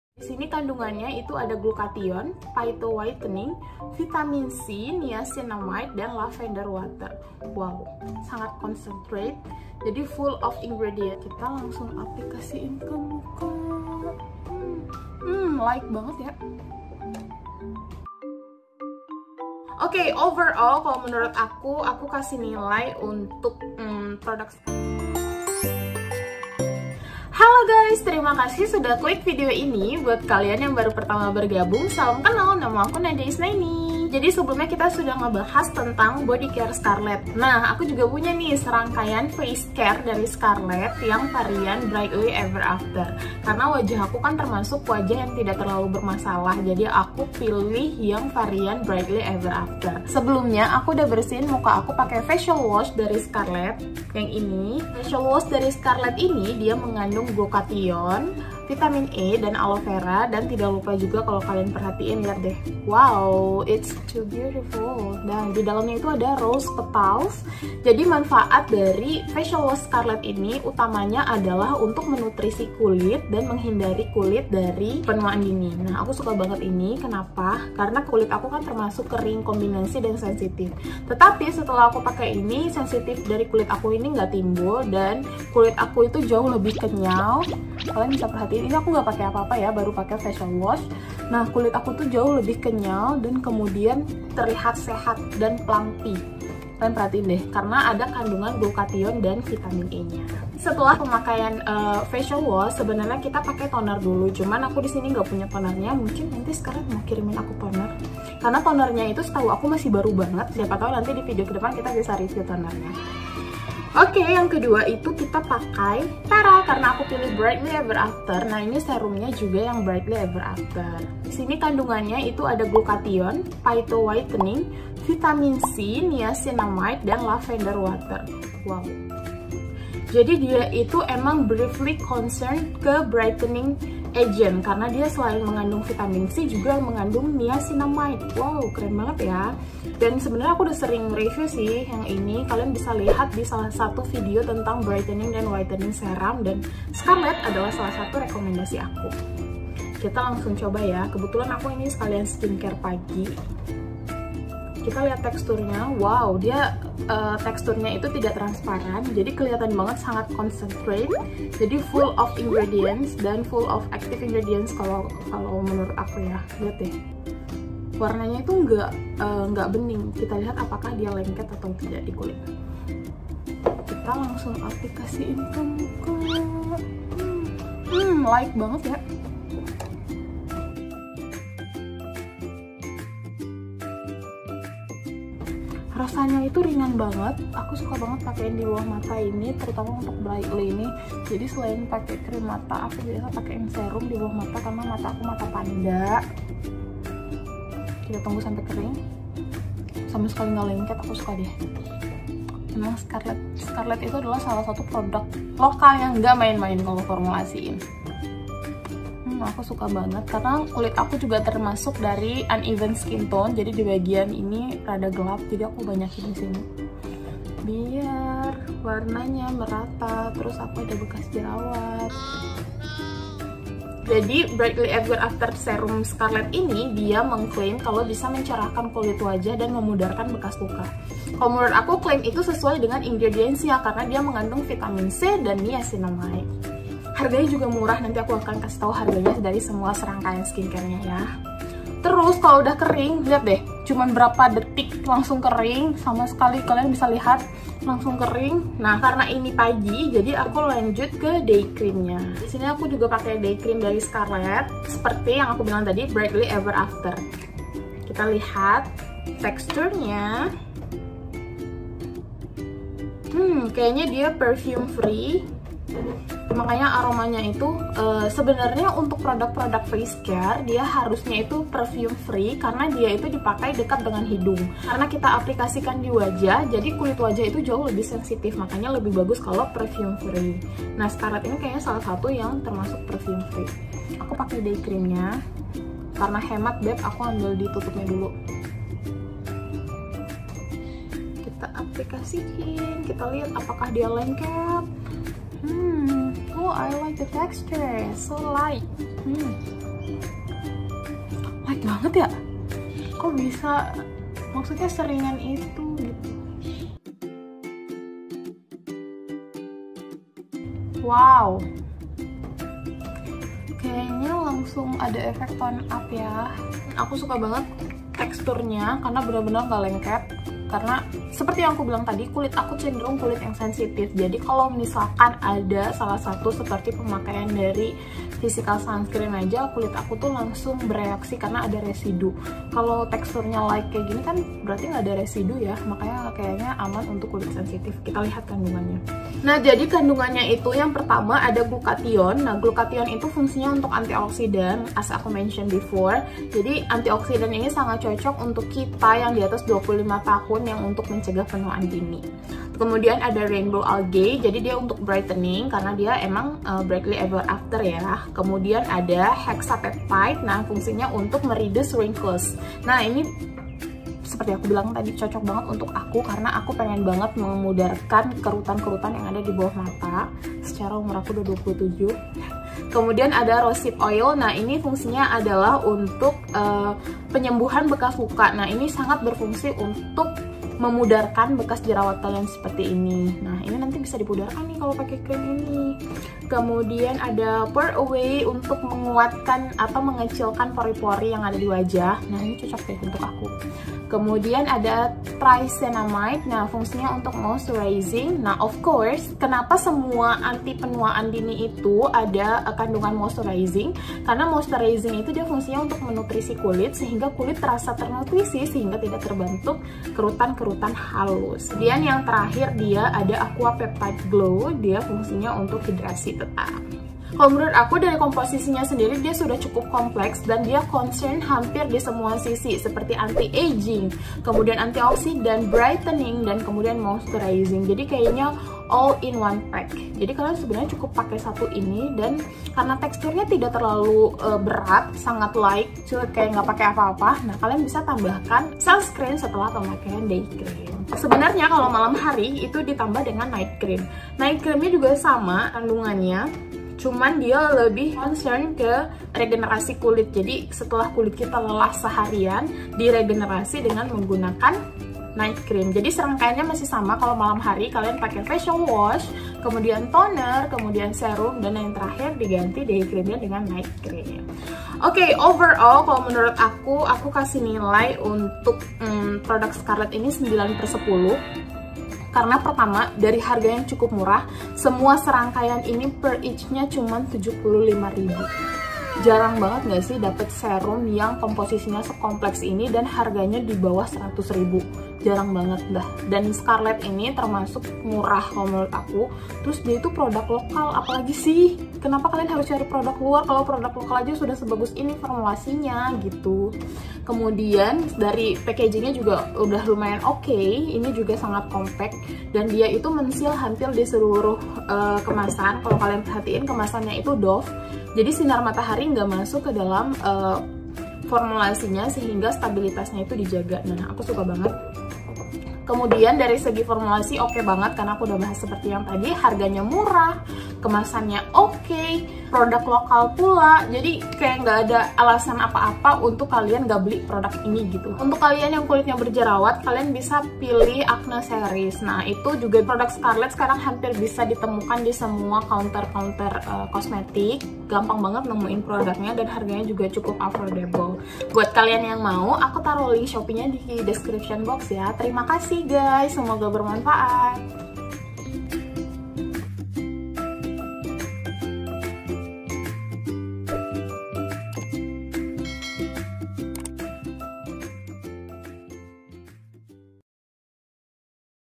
Step Pemakaian Face Care Scarlett Agar Hasilnya Maksimal
Review Face Care Produk Lokal: Scarlett Brightly Ever After. Setelah dipakai, ternyata beneran oke banget. Selain itu serangkaian produk scarlett ini juga termasuk dalam kategori skincare murah dan recommended banget deh!

2. Scarlett Serum Bightly Ever After
3. Scarlett Day Cream Brightly Ever After
4. Scarett Night Cream Brightly Ever After